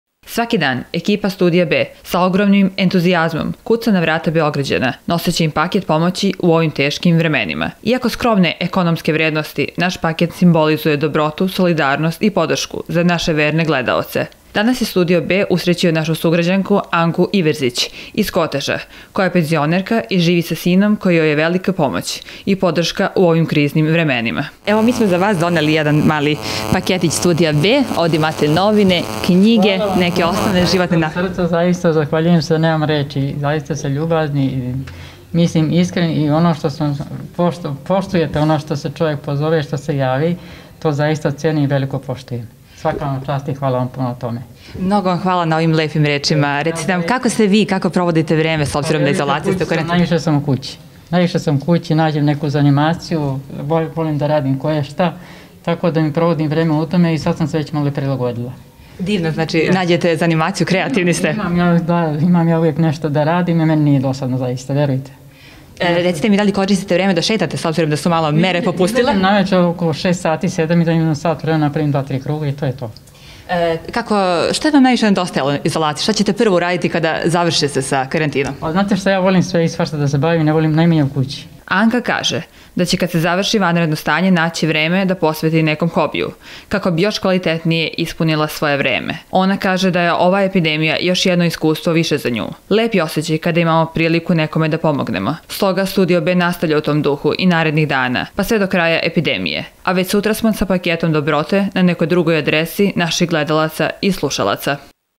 Humanitarna akcija Studija B
Studio B nastavlja sa humanitarnom akcijom u kojoj deli pakete onima kojima je  pomoć najpotrebnija. U borbi sa korona virusom najteže izlaze na kraj najstariji sugradjani, bolesni, kao i nasi gledaoci sa niskim prihodima, zato smo mi tu za njih. Ekipa Studija B posetila je našu vernu gledateljku iz Koteža  penzionerku Anku Ivezić.